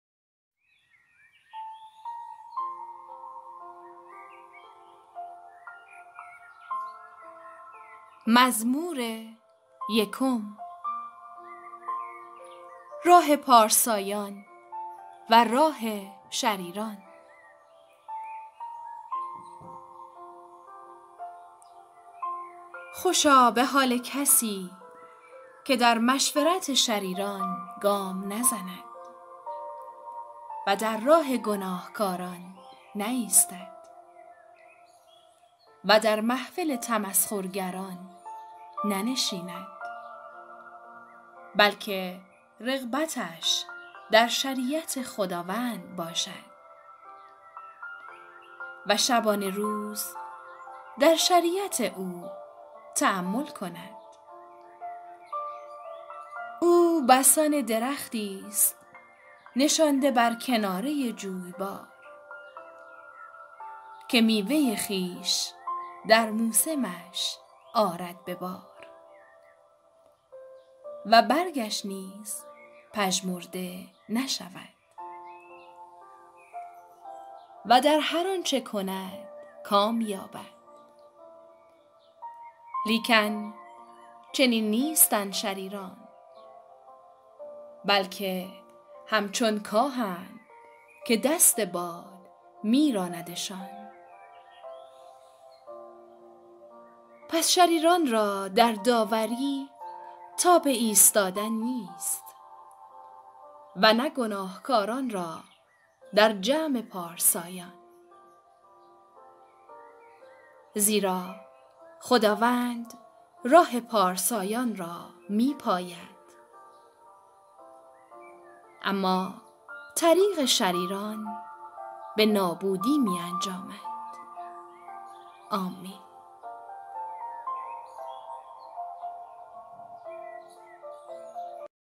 مزامیرباب اول آیات ۱تا۶راه پارسایان وراه شریران
باترجمه هزاره نو
خوشا به حال کسی که در مشورت شریران گام نزند و در راه گنهکاران نایستد و در محفل تمسخرگران ننشیند؛ بلکه رغبتش در شریعت خداوند باشد و شبانه‌روز در شریعت او تأمل کند. او بسان درختیست نشانده بر کنارۀ جویبار، که میوۀ خویش در موسمش آرد به بار، و برگش نیز پژمرده نشود، و در هر آنچه کند کام یابد. لیکن چنین نیستند شریران بلکه همچو کاهند که دستِ باد می‌رانَدشان. پس شریران را در داوری تابِ ایستادن نیست و نه گنهکاران را در جمع پارسایان. زیرا خداوند راه پارسایان را می‌پاید اما طریق شریران به نابودی می‌انجامد.